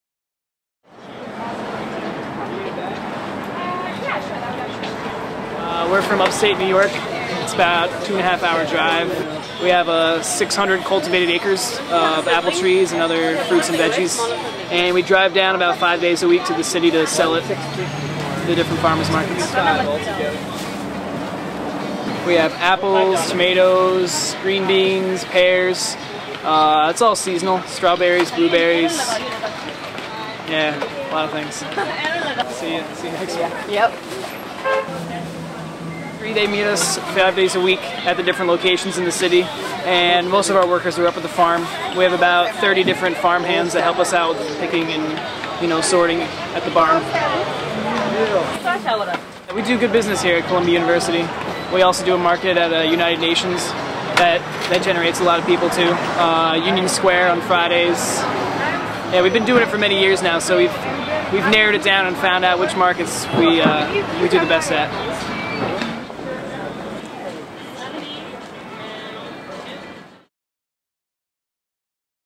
Green Market@Morningside Heights
The Green Market in front of Columbia University is welcoming everyone every Thursday and Sunday on Morningside Heights. The food is fresh and delicious.